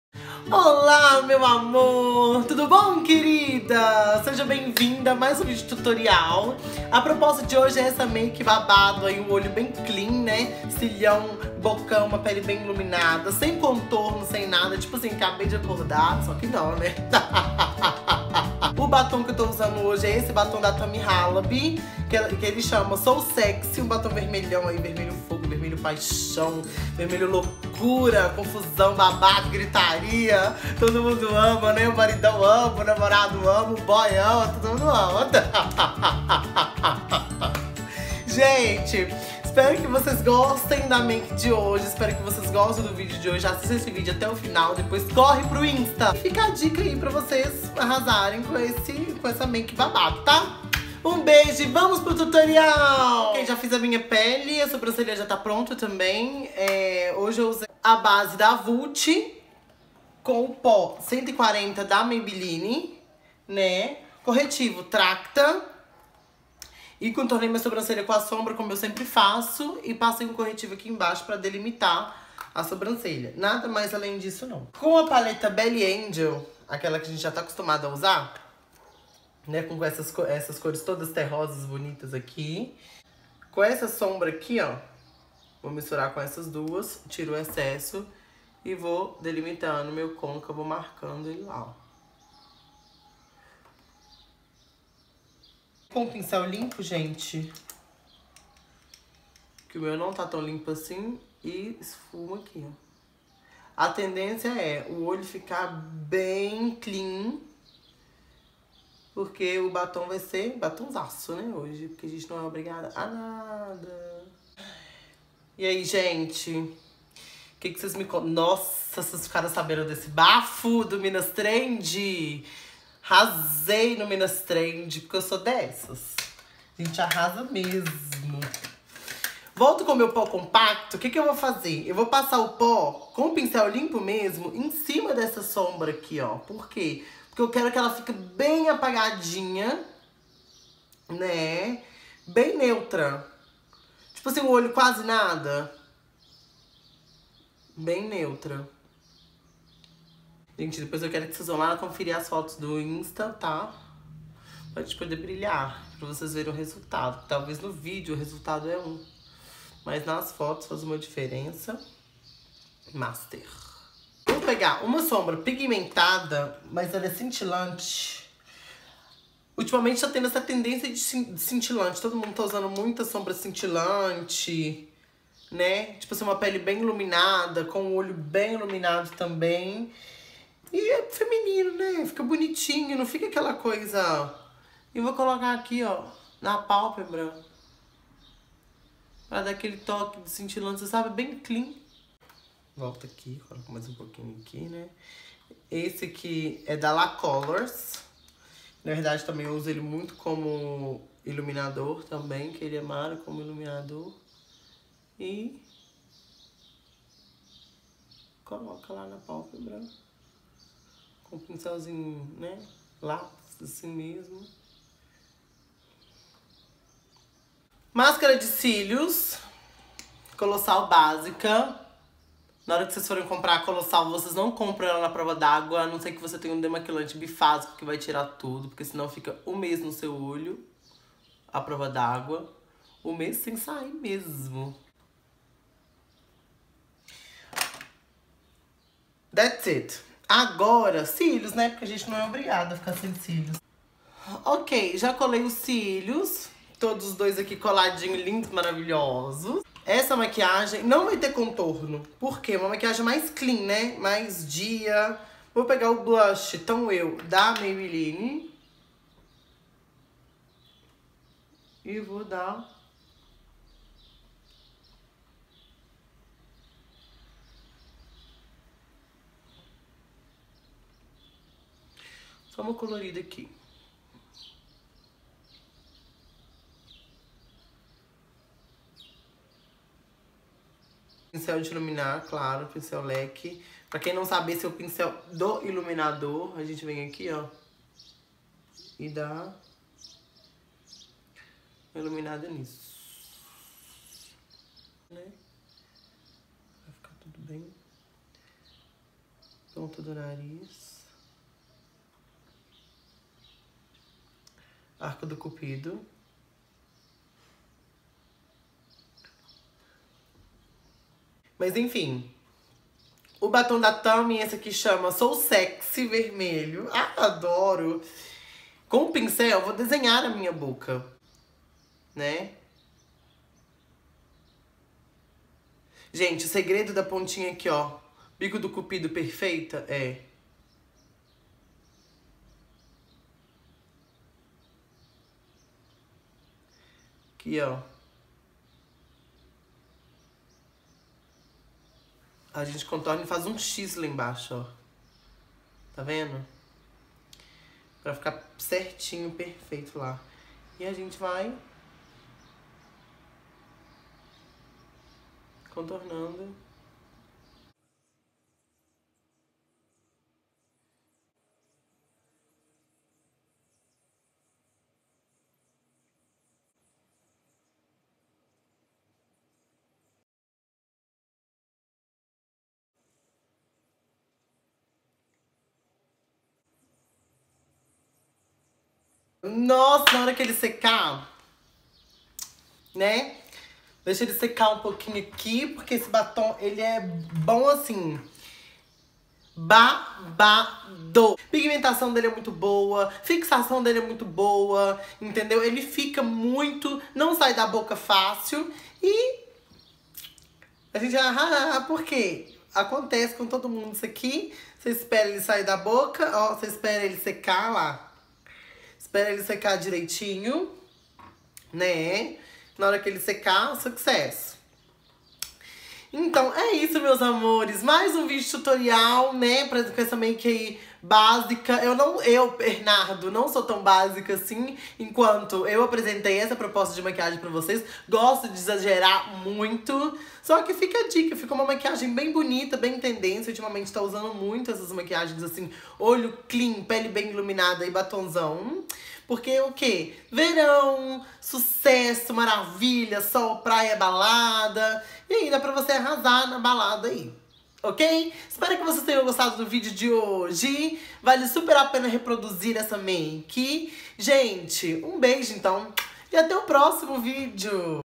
COMO FAZER UMA MAQUIAGEM SIMPLES COM CARA DE SOFISTICADA POR BERNARDO LETRO
Oiiii pessoas... tudo bom com vocês ?
Mais um tutorial no AAAAR!
Dessa vez uma make super básica mas bem sofisticada!
Com esse batom maravilhoso da nossa querida @tamihalabi! Sou sexy!
Aproveitem o tutorial depois passa lá no insta para ver o resultado nas fotos!